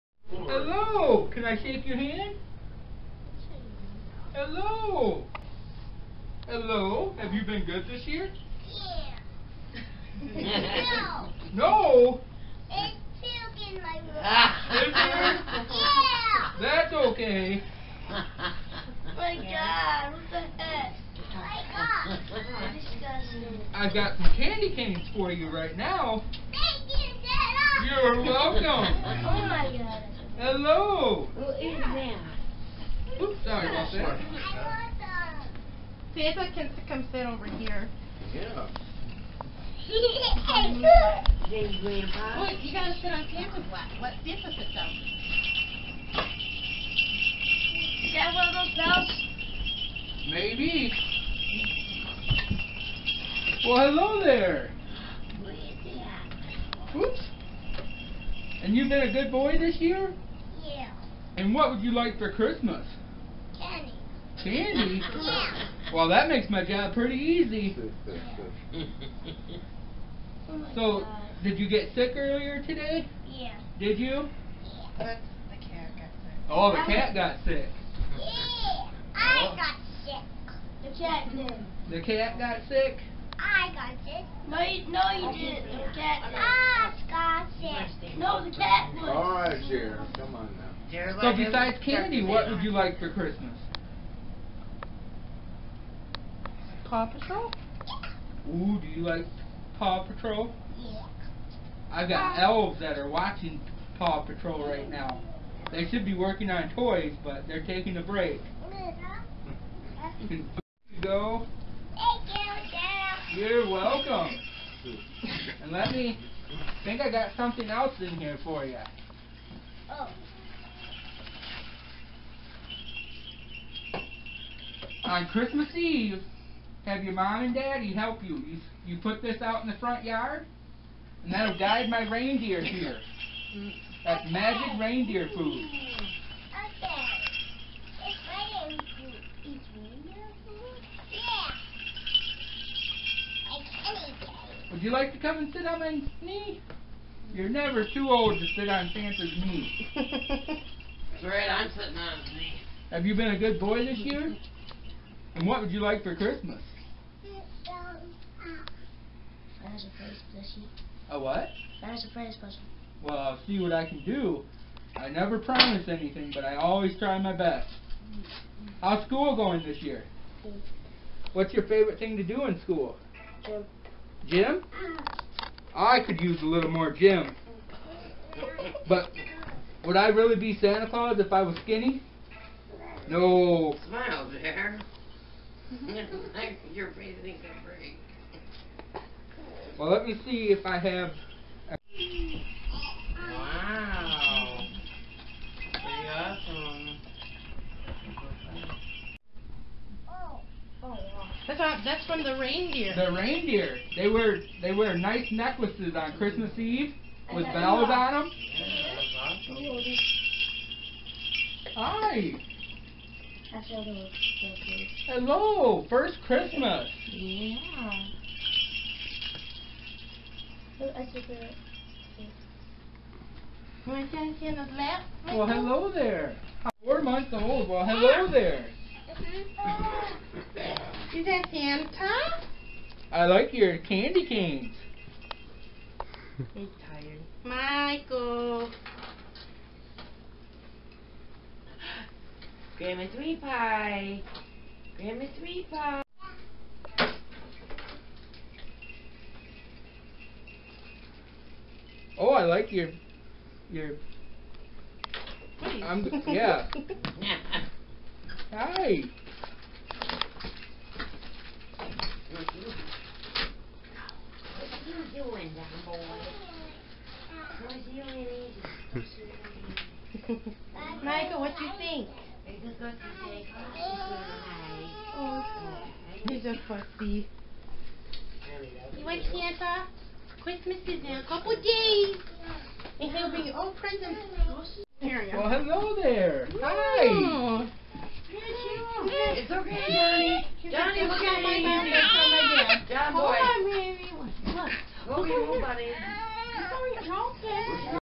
Santa Shows Up
Sitting around the house enjoying the day till we hear bells ringing outside and all of a sudden Santa walks in the door to surprise us.